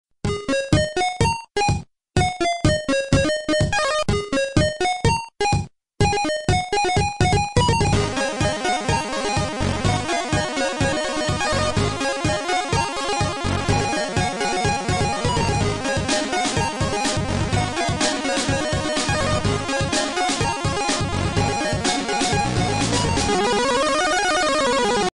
[MMD French] My little Pony : Just in bit (meme)
[Motion in coming]

Et ouais je fais les memes version Poney pour les fanatiques de mikumikudance ET My little pony :) je vous souhaite bon visionnage.